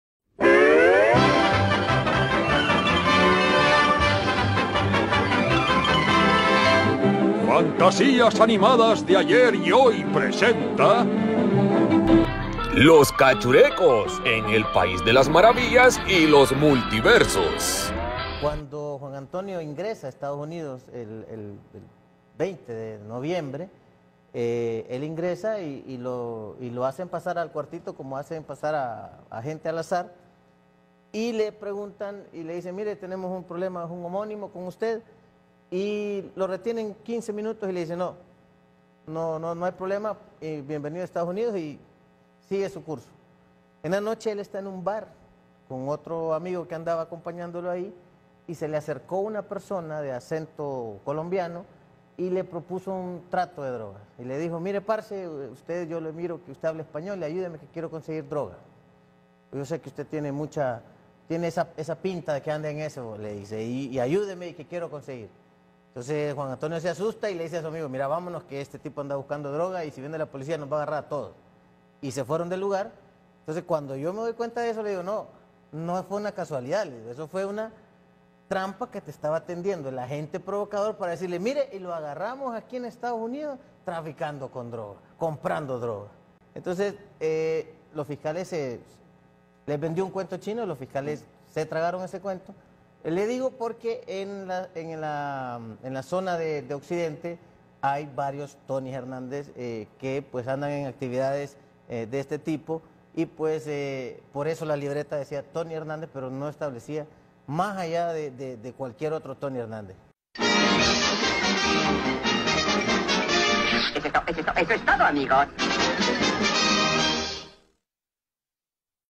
Narco-Dictadura-Honduras, Narco partido nazional sigue negando delitos
Segun los politicos y simpatizantes del narco cartel nazionalista, todos los ciudadanos de Honduras, somos imbeciles como sus seguidores, ellos piensan que todos creemos en sus narco-novelas y teorias de conspiracion... SON DELINCUENTES, SON NARCOS, SON CORRUPTOS!